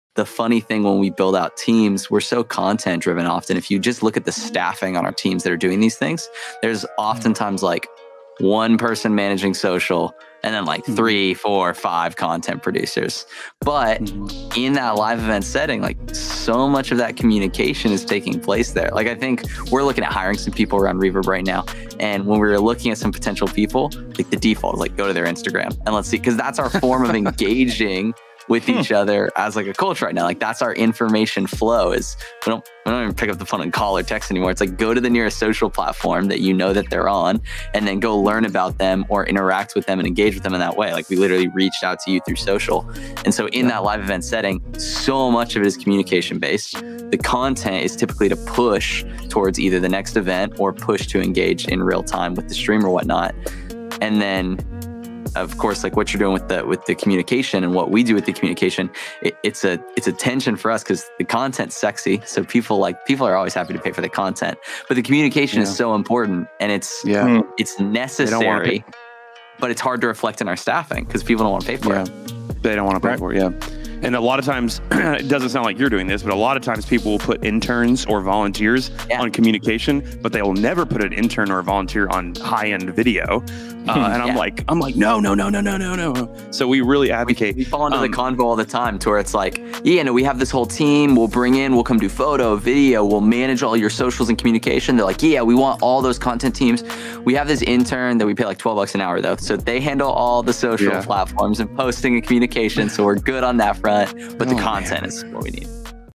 Stop Putting Interns In Charge of Your Communications / RVRB Round Table
RVRB Round Table
Why should you stop putting interns in charge of your communications? Quite simply you're communication and social interaction with your audience is the most important part of your brand. The person in charge should both know your brand and understand the importance of communication.